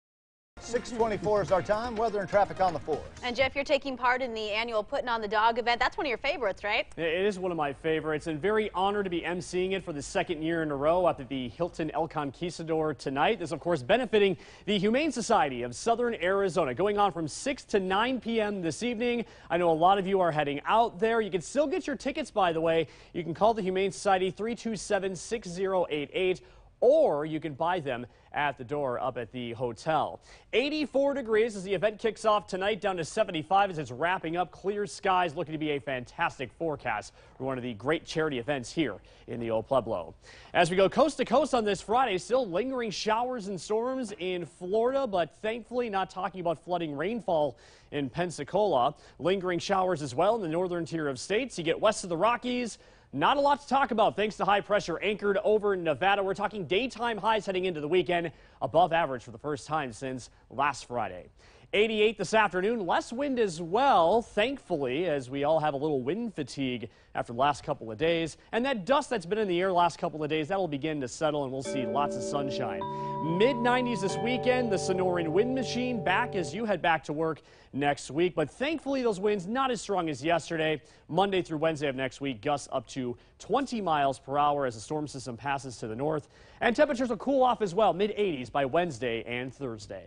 Beamish Blogcast: May 2nd, 2014
Tucson weather forecast for May 2nd, 2014, from News 4 Meteorologist Jeff Beamish.